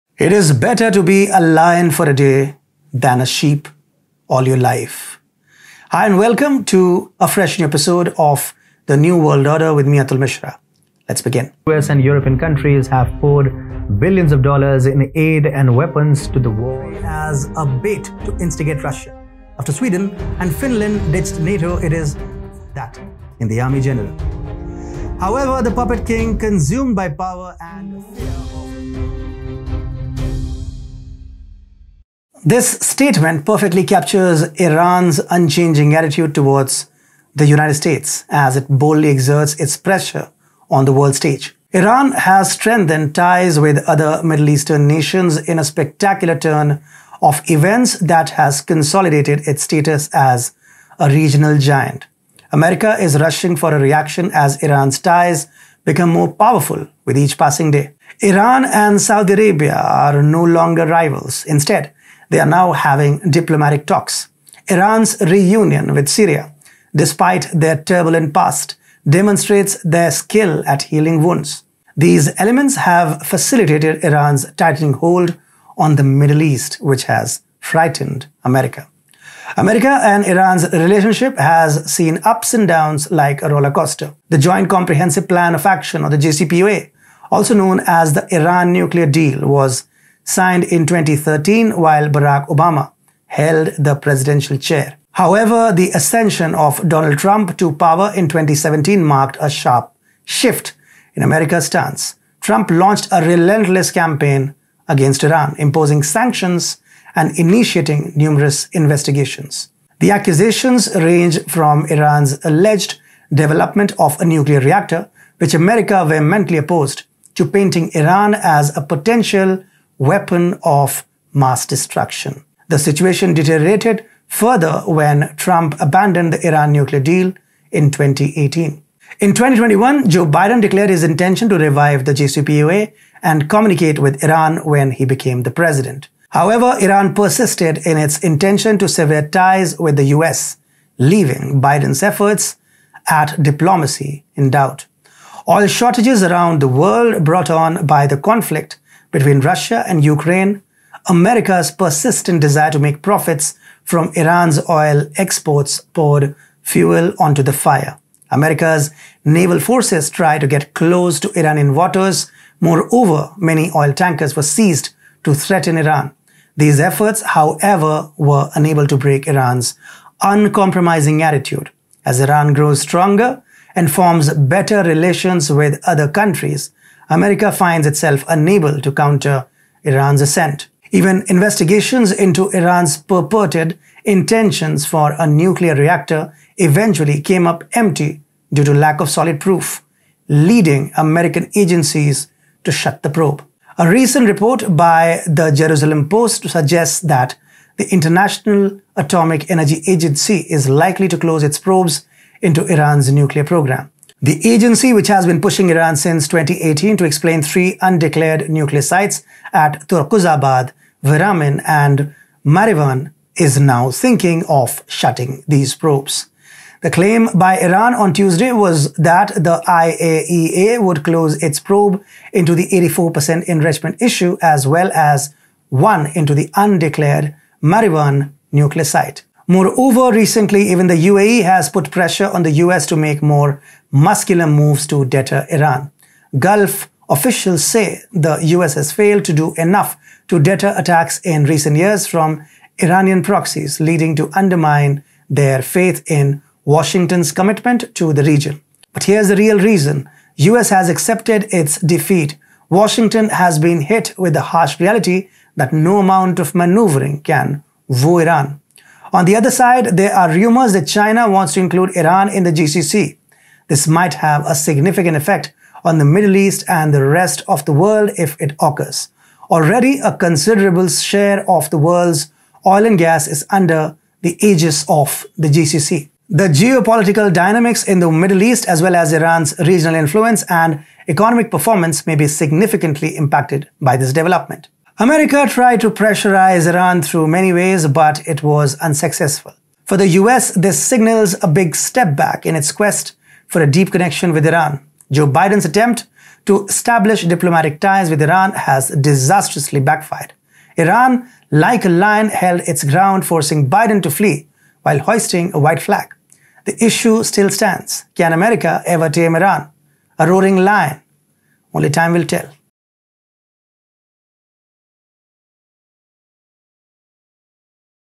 TheNewWorldOrder : It’s Official: Biden is waving white flag against Iran
From shattered deals to futile pressure tactics, Biden's attempts to tame Iran crumble as the lion of the Middle East stands tall. Iran's unyielding power leaves White Flag waving, shattering US hopes of taming its regional influence. A battle for dominance ensues.